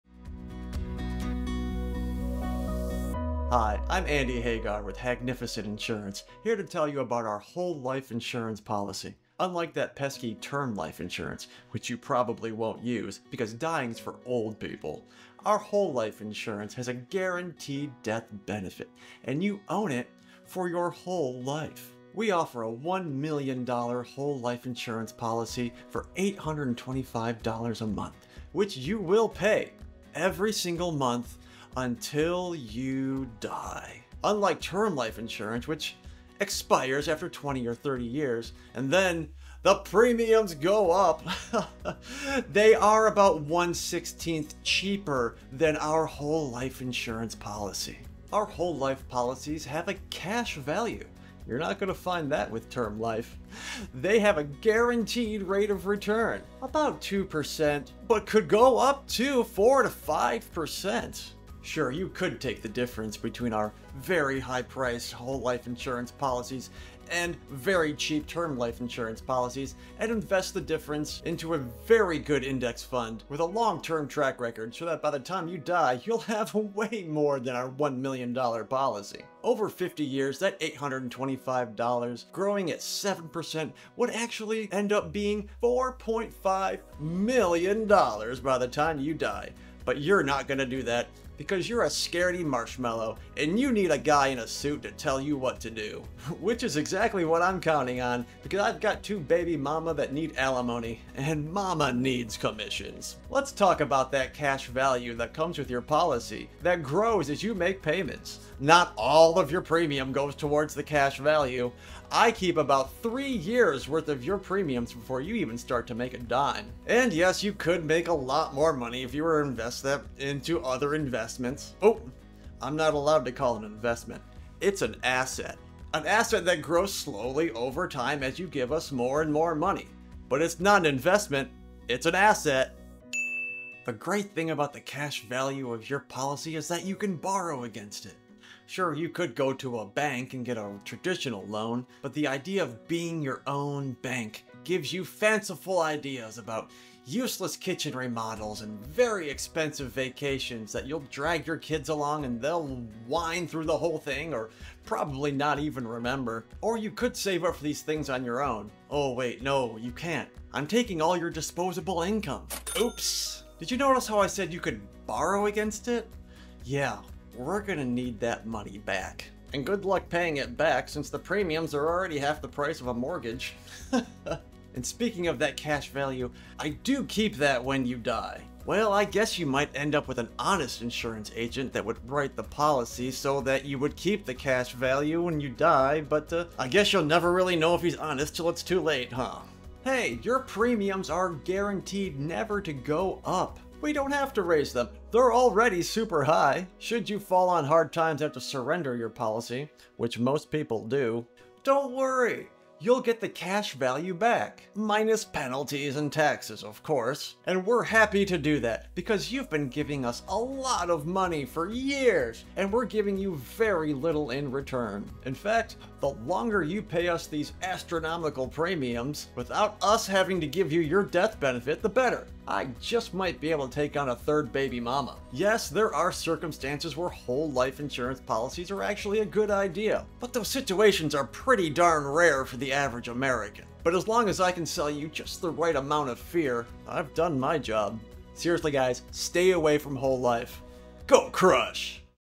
What's Whole Life Insurance Hiding? (Parody)
Get ready to LOL as we expose the truth about whole life insurance in this hilarious infomercial parody! From confusing policy terms to outrageous premiums, we're diving into the wild world of whole life insurance and bringing back the laughs. Think you know what you're getting yourself into with whole life? Think again! Join us as we poke fun at the industry and bring some much-needed transparency to the world of insurance. So sit back, relax, and get ready to have your mind blown (and your funny bone tickled) in this unforgettable parody!